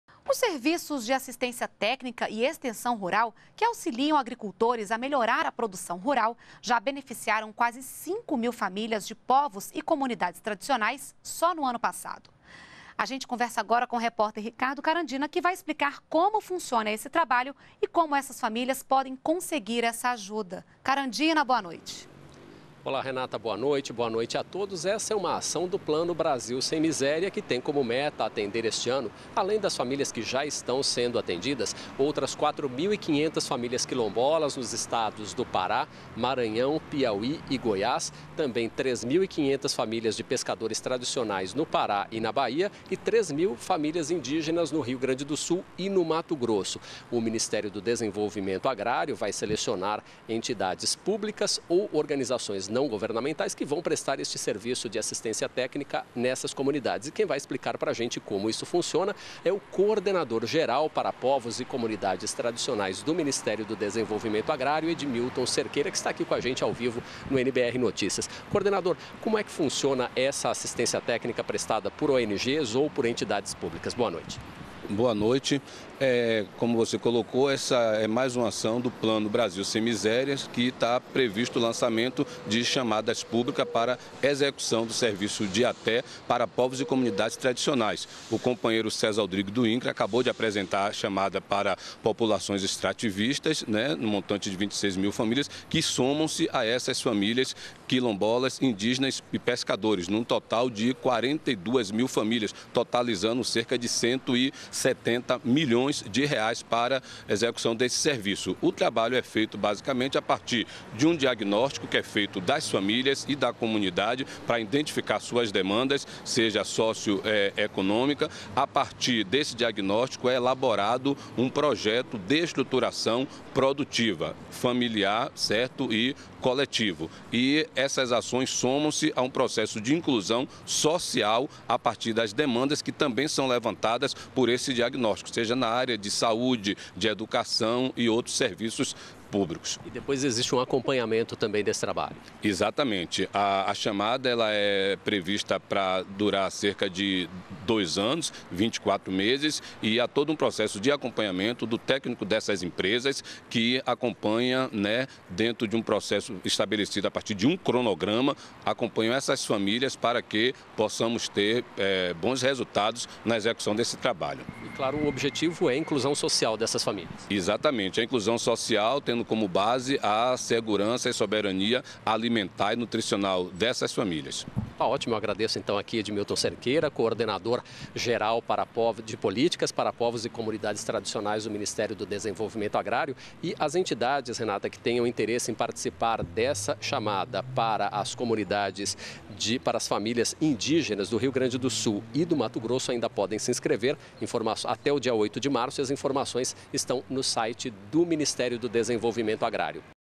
Brasil Sem Miséria beneficiou 5 mil famílias de povos e comunidades tradicionais em 2012
NBR NOTÍCIAS - 31.01.13: Os serviços de Assistência Técnica e Extensão Rural (ATER) beneficiou quase 5 mil famílias quilombolas, indígenas e extrativistas em 2012. Neste ano, o Ministério do Desenvolvimento Agrário (MDA) vai selecionar entidades públicas e organizações não governamentais para prestarem esses serviços a agricultores e melhorar a produção rural. O programa faz parte do Brasil Sem Miséria. O NBR Notícias entrevistou o coordenador de Políticas para Povos e Comunidades Tradicionais do MDA, Edmilton Cerqueira, sobre as oportunidades oferecidas em 2013. Mais informações também estão disponíveis no